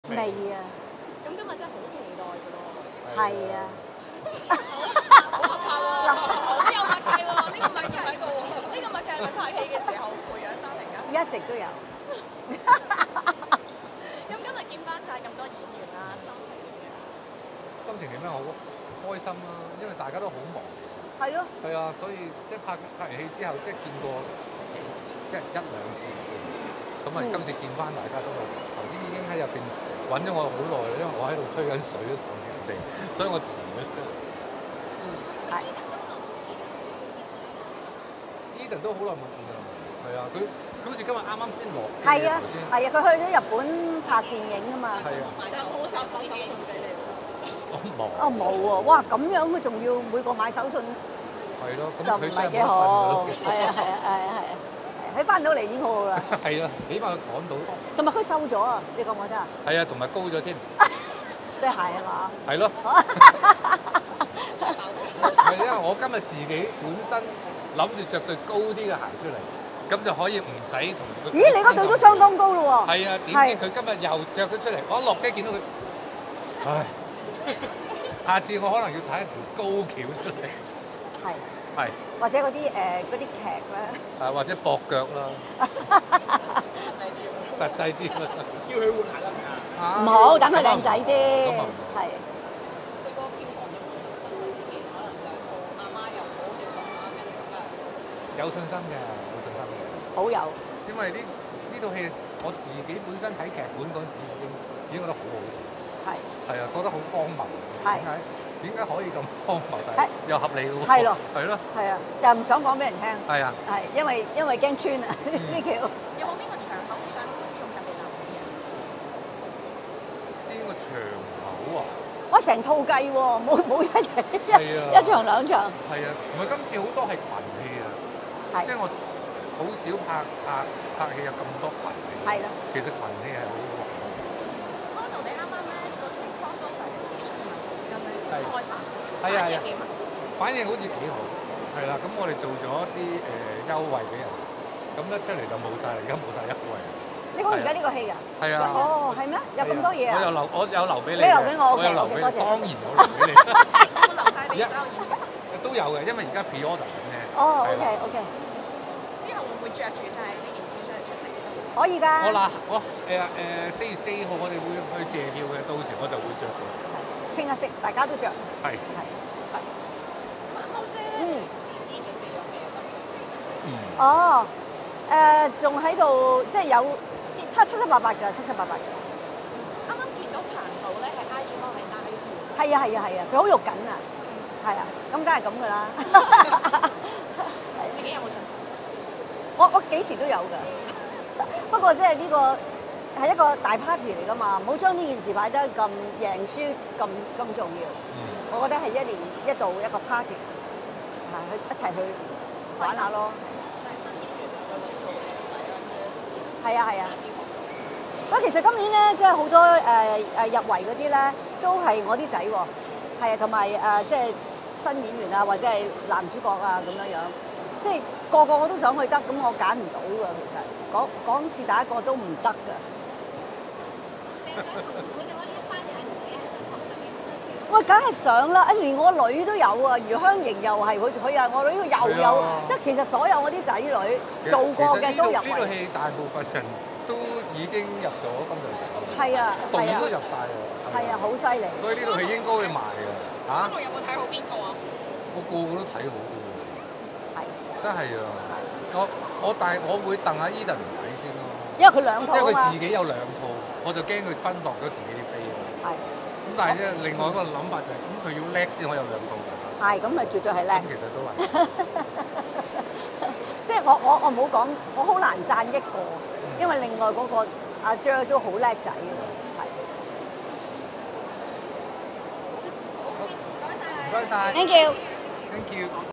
死屍死時四十四｜鄭中基爆Edan呂爵安著增高鞋 毛舜筠唔介意冇手信
由毛舜筠(毛姐)、鄭中基、呂爵安(Edan)及柳應廷(Jer)等主演的《死屍死時四十四》今日(29日)舉行首映。毛姐與鄭中基表示很開心可以與一眾演員再見面，坦言大家都很忙，一直沒有時間相聚。二人又表示對《死》片票房成績充滿信心，鄭中基稱看劇本時已覺得好笑又荒謬，但現在不便劇透。他們更透露下月4日會一齊亮相謝票，並會穿上相關聯乘產品現身。

提到Edan從日本回港卻沒有手信，二人表示沒有介意，毛姐說︰「仲要咁樣每個買手信就唔係幾好，返到嚟已經好好！」毛姐笑言感覺Edan消瘦了，而鄭中基就覺得對方又瘦又高，但他與毛姐所指的「高」，是形容Edan的鞋。鄭中基自言今日專登穿了比較高的鞋出來，沒想到Edan所穿的比他還要高，「下次要踩高橋同Edan對撼」。

毛姐入圍角逐金像獎影后，她表示一直對自己充滿信心，亦很開心曾與她合作的年輕演員均獲提名，她稱無論是Edan、Jer還是余香凝，他們的表現都很好，難以比較誰較好。

bmgproductionmusic